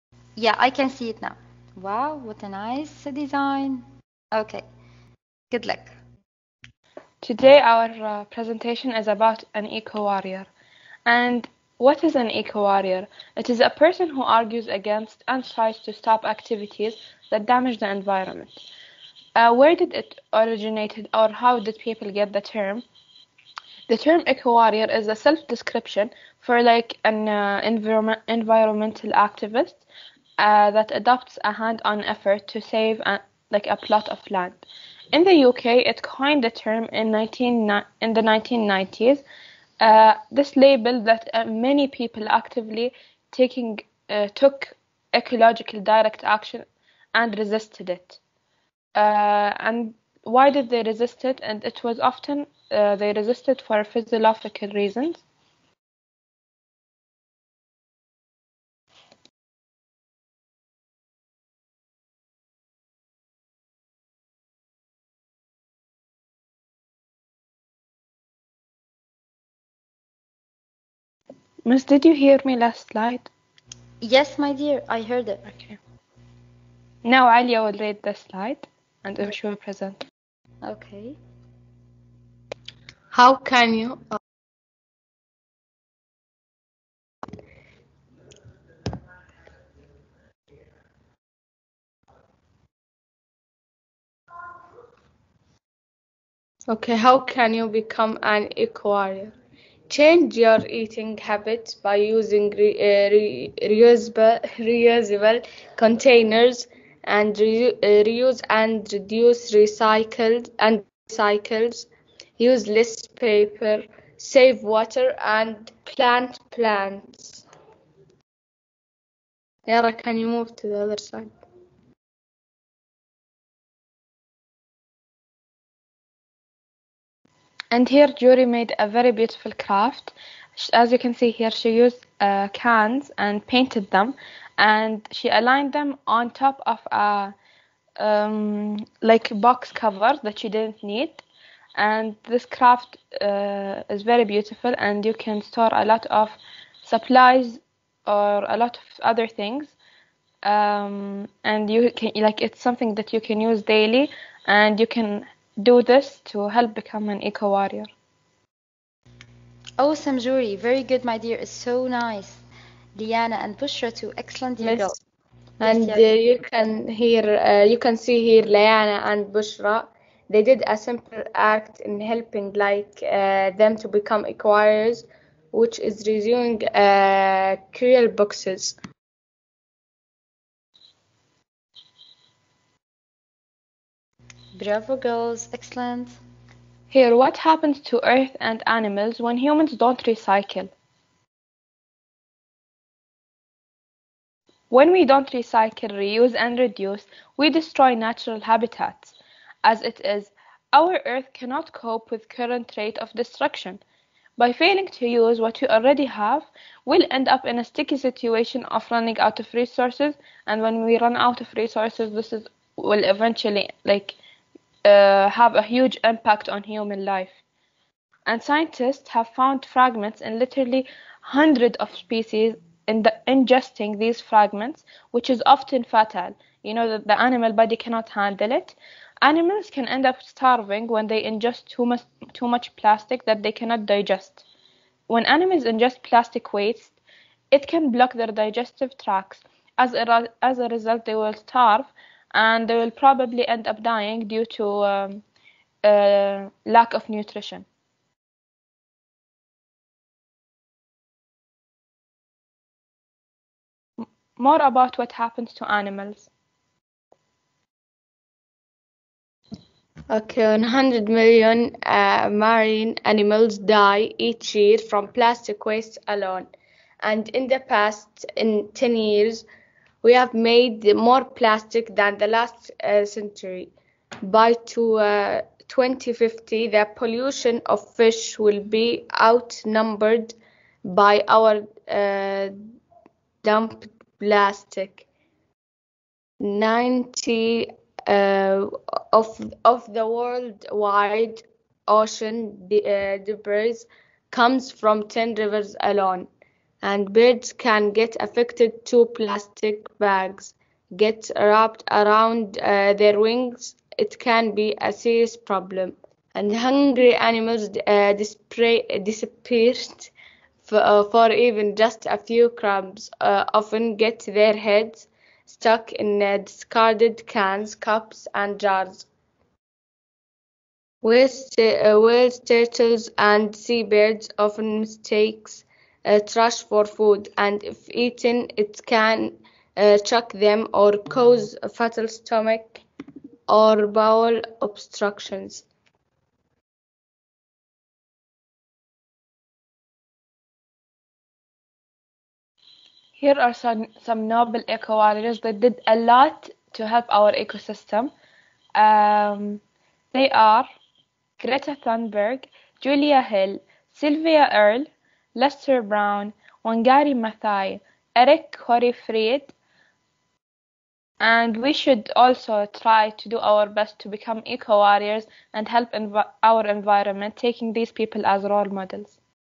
Gr 9 chemistry project
AHISR Grade 9 American had a wonderful project on “ Eco warrior” . In the purpose of guiding people to save the Earth during their Chemistry Virtual Session.